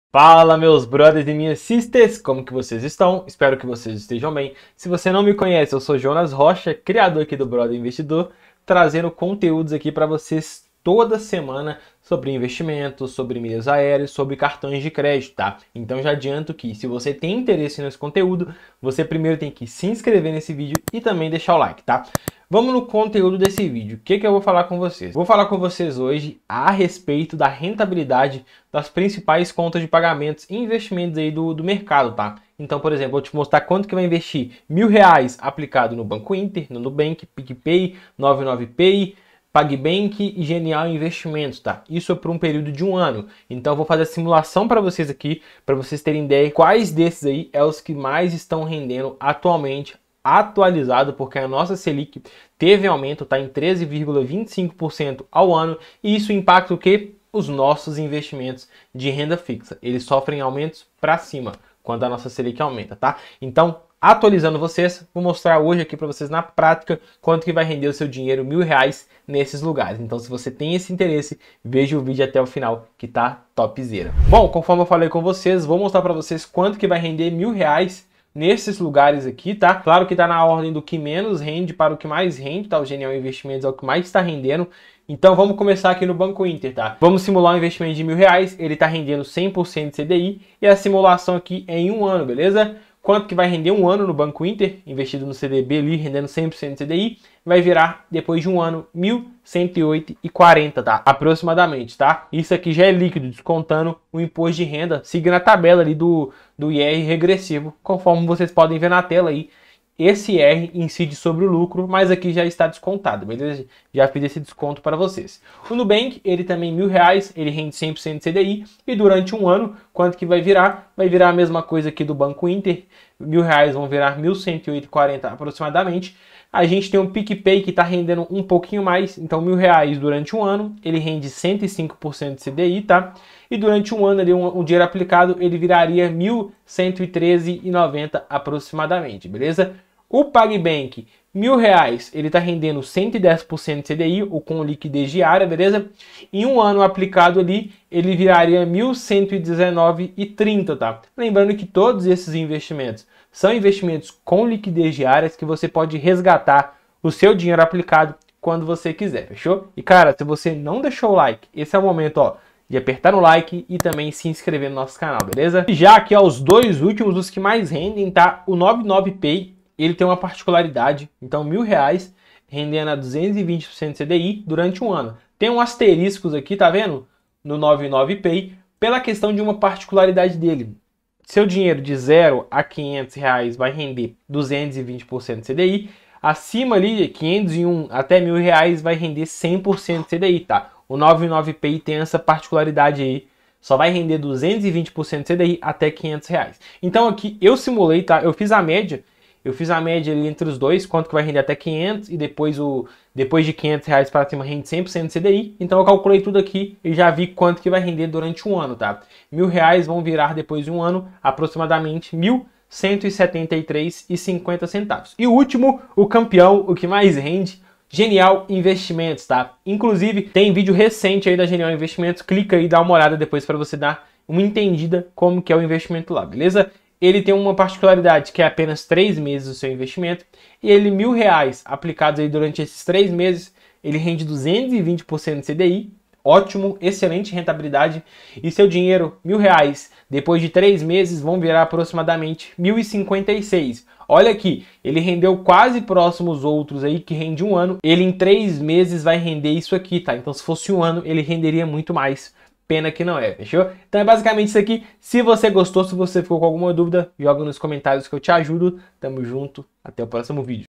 QUANTO VAI RENDER R$ 1.000,00 NESSES INVESTIMENTOS?
Fala meus Brothers e Sisters, como estão?

Nesse vídeo eu venho mostrar a vocês quanto rende R$ 1.000,00 nesses investimentos, após o aumento que ocorreu na nossa SELIC paraa 13,25%.
Com esse aumento os investimentos passaram a render mais, esse vídeo já é o atualizado.